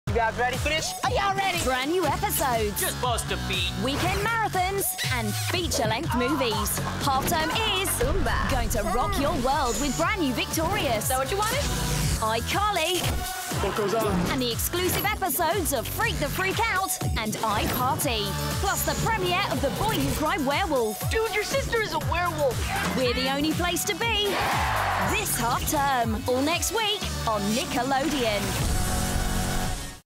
Nickelodeon_Halfterm_Highlights_30
Here is a job I did for Nickelodeon. It's basically a run-down of all their Half term Shows and Movies. The graphics were inspired by the kinds of scribbles and doodles kids do in their exercise books at school.